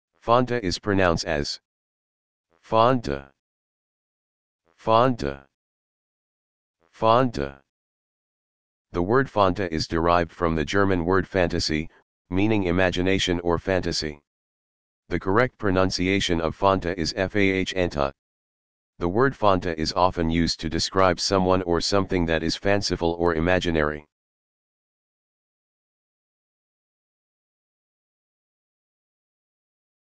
How To Pronounce Fanta
The word "fanta" is derived from the German word "fantasie," meaning "imagination" or "fantasy." The correct pronunciation of "fanta" is "FAHN-tuh." The word "fanta" is often used to describe someone or something that is fanciful or imaginary.